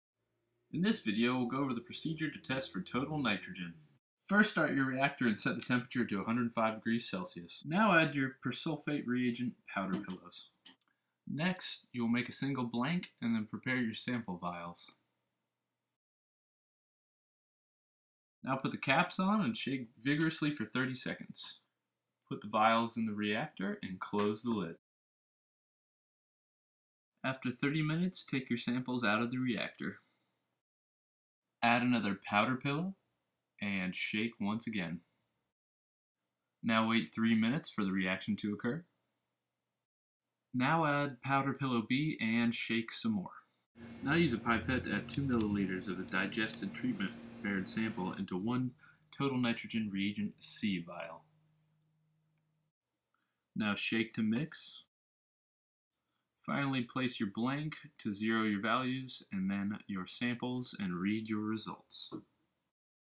Total Nitrogen
Method of measuring Total Nitrogen in lab